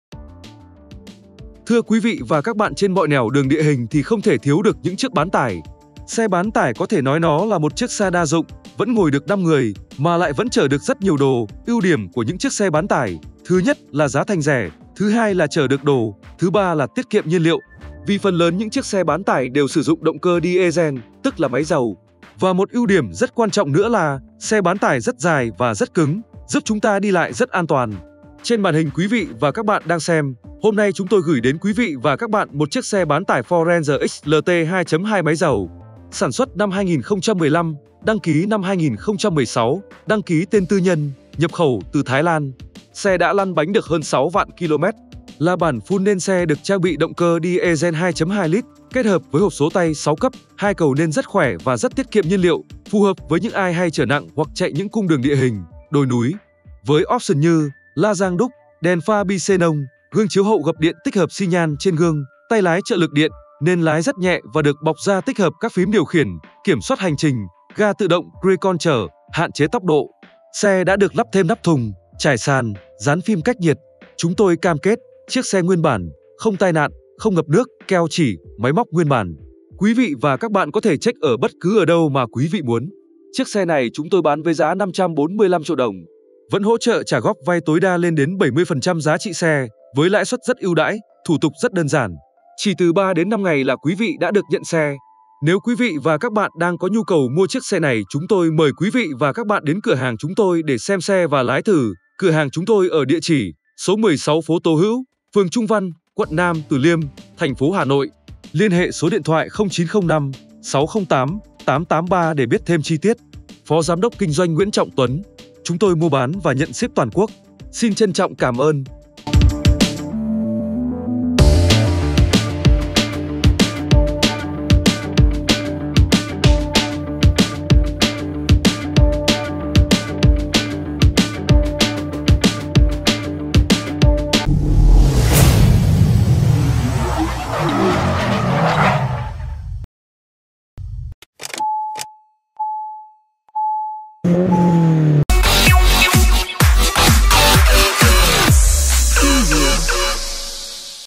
Báo giá ngay xe Ford Ranger XLT 2.2, 2 cầu 2016 bản full, tay lái trợ lực điện giá chỉ 545tr
Bán xe Ford Ranger XLT 2.2 máy dầu sản xuất năm 2015, đăng ký năm 2016, đăng ký tên tư nhân,nhập khẩu Thái Lan. Đã chạy được 6,5 vạn km.
Bản full xe được trang bị động cơ diesel 2.2 L, hộp số tay 6 cấp, 2 cầu nên rất khỏe và rất tiết kiệm nhiên liệu, phù hợp với những ai hay chở nặng hoặc chạy những cung đường địa hình, đồi núi. Option : lazang đúc, đèn pha bi xenon, gương chiếu hậu gập điện tích hợp xi nhan trên gương, vô lăng bọc da tích hợp các phím điều khiển, kiểm soát hành trình, ga tự động , hạn chế tốc độ.
Đã lắp thêm nắp thùng, trải sàn, Fim cách nhiệt...
Xe đẹp nguyên bản
Hỗ trợ trả góp